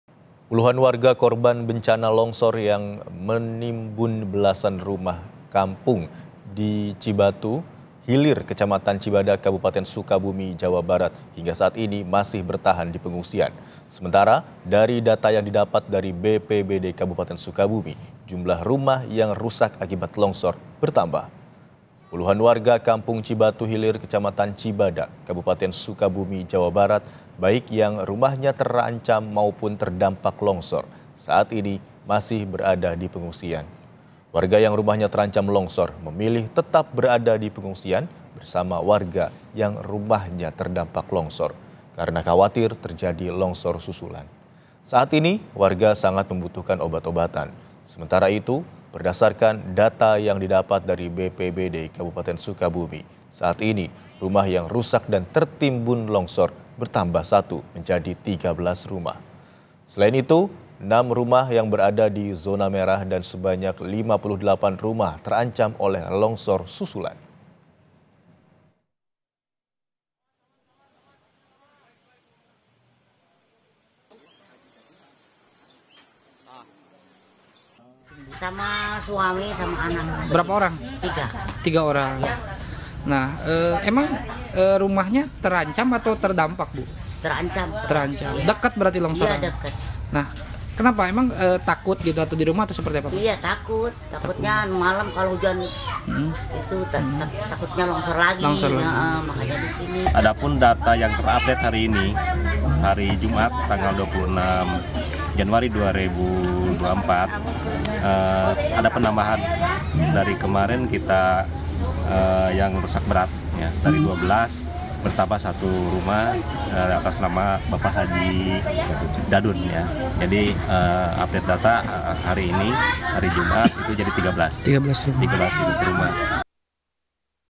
Was-Was, Warga di Sukabumi yang Rumahnya Terancam Longor Pilih Menetap di Pengungsian
SUKABUMI, KOMPAS.TV - Puluhan warga korban bencana longsor yang menimbun belasan rumah di Kampung Cibatu Hilir, Kecamatan Cibadak, Kabupaten Sukabumi, Jawa Barat hingga saat ini masih bertahan di pengungsian.

Sementara dari data yang didapat dari BPBD Kabupaten Sukabumi, jumlah rumah yang rusak akibat longsor bertambah.

Warga yang rumahnya terancam longsor memilih tetap berada di pengungsian bersama warga yang rumahnya terdampak longsor karena khawatir terjadi longsor susulan. Saat ini warga sangat membutuhkan obat obatan.

Data BPBD Kabupaten Sukabumi, rumah rusak dan tertimbun longsor bertambah 1, total menjadi 13 rumah. Selain itu, 6 rumah berada di zona merah dan sebanyak  58 rumah terancam oleh longsor susulan.

Baca Juga Longsor di Wonosobo Menyeret Mobil dan Merusak Rumah Warga di